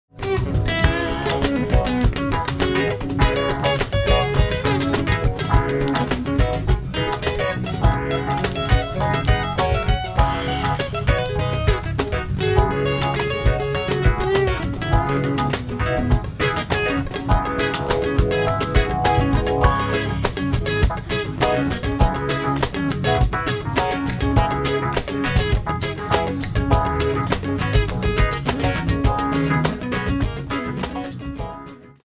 Akustik Band Bayern | 4Tex Unplugged
4Tex Unplugged live beim grooven. Die Akustik Band aus Bayern für Euer Live-Event.